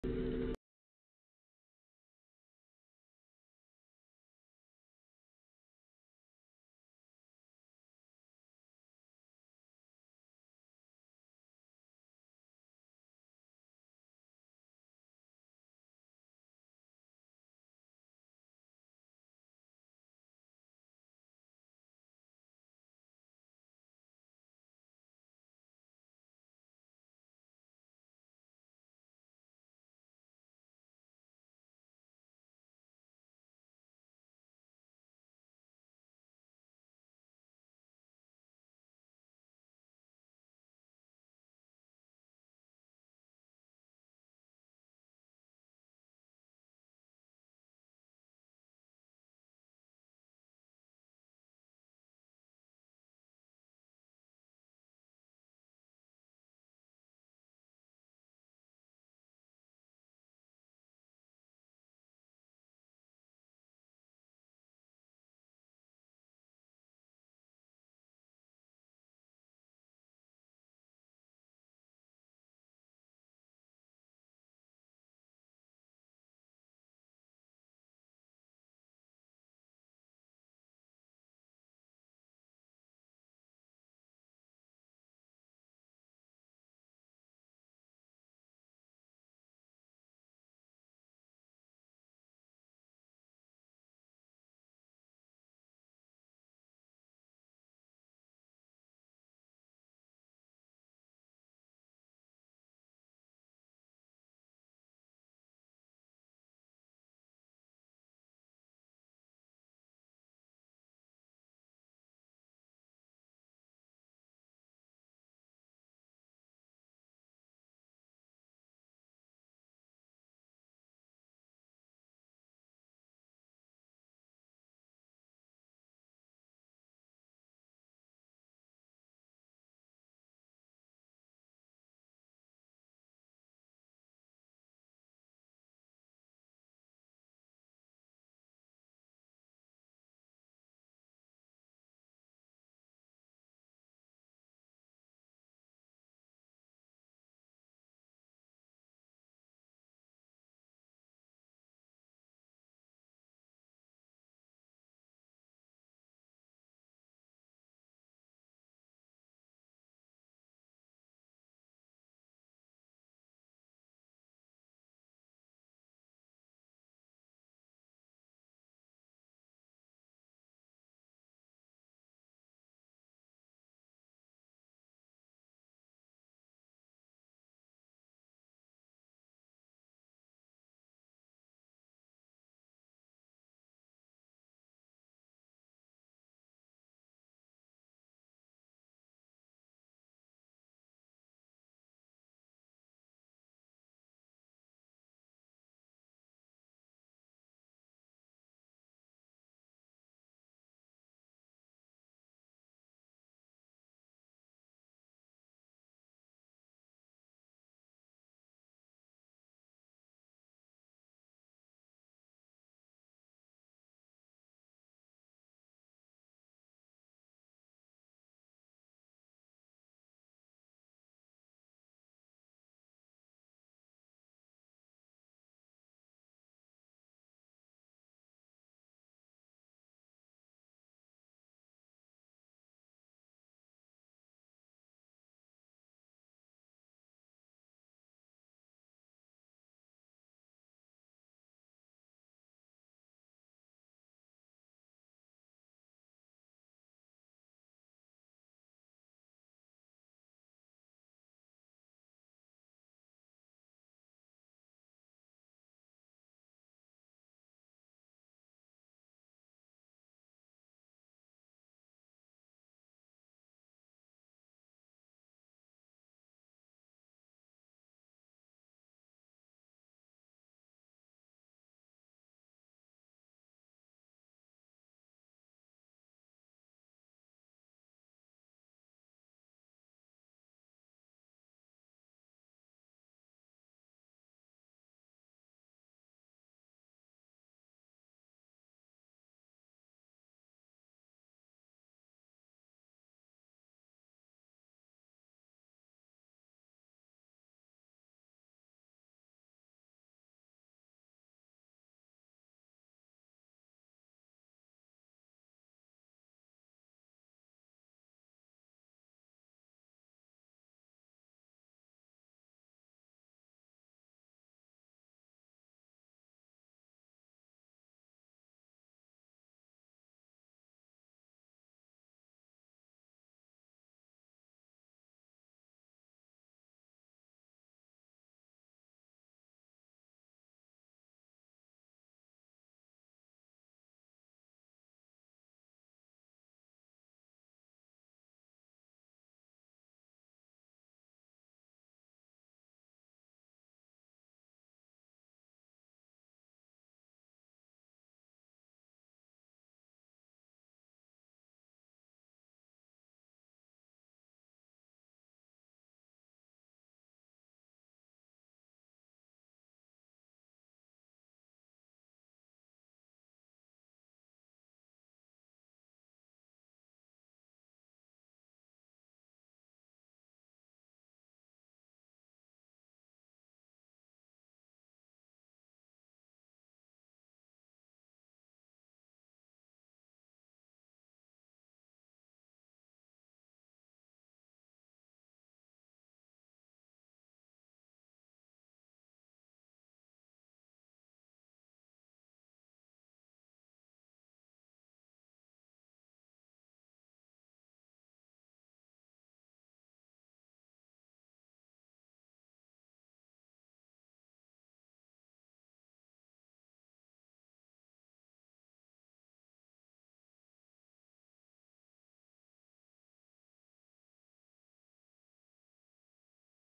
Otobüste Kalp Krizi Geçiren Vatandaşı Şoförün Hızı Kurtardı
Sivas'ta özel halk otobüsü içerisinde kalbi durarak aniden oturduğu koltuktan düşen Sadettin Tosun'u şoför Serkan Y., en yakın aile sağlığı merkezine yetiştirdi. Buradan da Numune Hastanesi'ne kaldırılan Tosun, tedavi altına alındı. Olay anı ise otobüsün güvenlik kamerasına yansıdı.